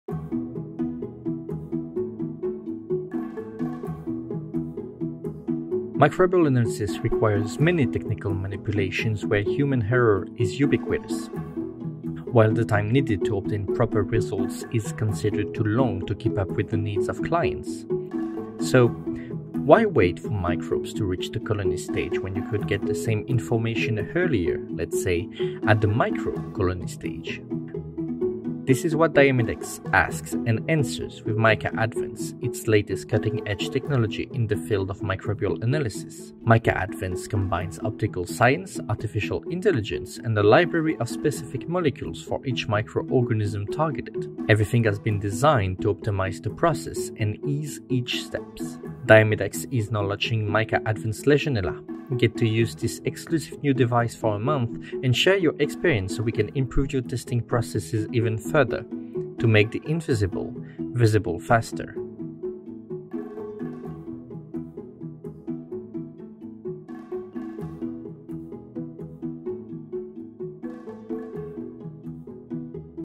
MICA Advance Legionella by DIAMIDEX
Be the first Microbiological Laboratory to try out the next step in Legionella pneumophila control for a month. DIAMIDEX opens out MICA Advance to try outs, use the best there is in Legionella pneumophila testing and share your feedback to improve your processes even further.

MICA Advance is the latest counter by DIAMIDEX, optimized to reduce handling time, human error, consumable use and vastly decrease the process time to 48 hours only. The Legionella pneumophila solution is as easy to use as it is efficient and allow to perform until 500 analysis a day !

Credits :
Visual element : cursor by Feri Saputra – The Noun Project